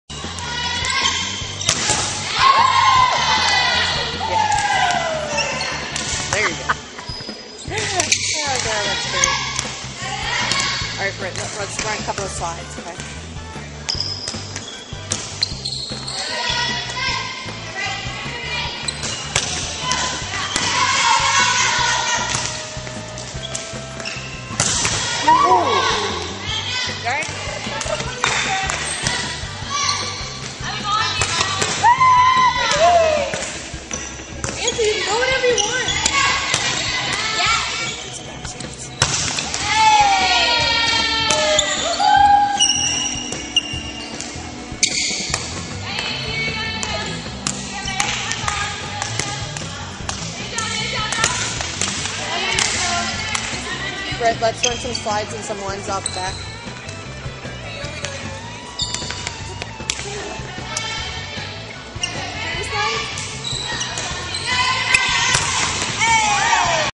VOLLEYBALL RECRUITING VIDEO: Setter Skills Example
Setters are one of the hardest positions for a coach to recruit. They are looking for talent, a leader, a jumper and someone who is confident. Here is an example of a skills video for a setter. Setters can break up their skills videos by position, jump-setting only, quick sets, etc. or can put them all in one video. Coaches aren't picky and just want to see what you can do!